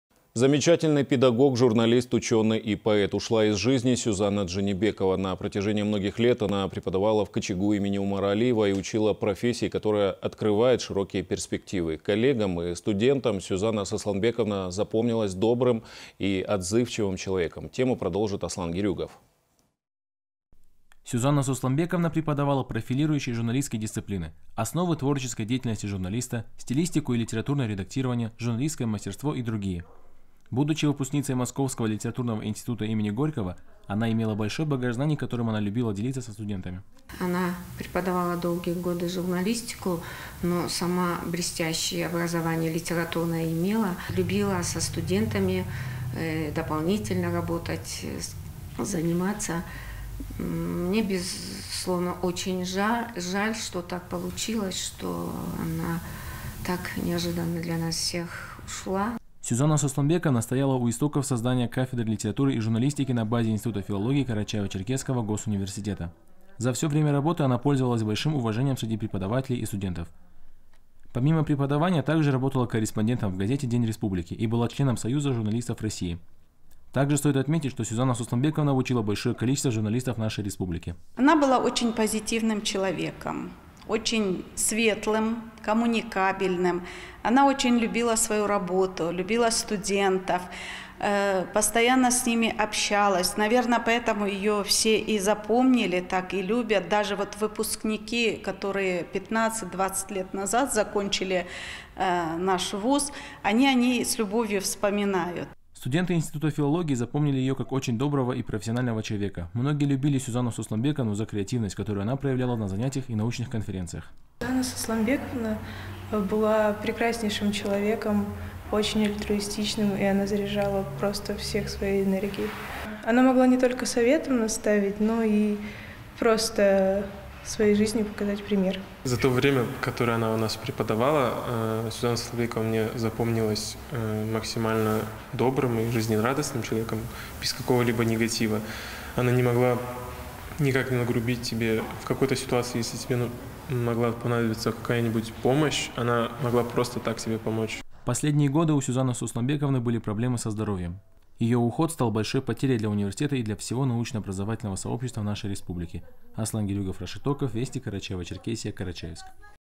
Замечательный педагог, журналист, учёный и поэт: ушла из жизни Сюзанна Джанибекова
Замечательный педагог, журналист, учёный и поэт: ушла из жизни Сюзанна Джанибекова. На протяжении многих лет она преподавала в КЧГУ имени Умара Алиева, и учила профессии, которая открывает широкие перспективы. Коллегам и студентам Сюзанна Сосланбековна запомнилась добрым и отзывчивым человеком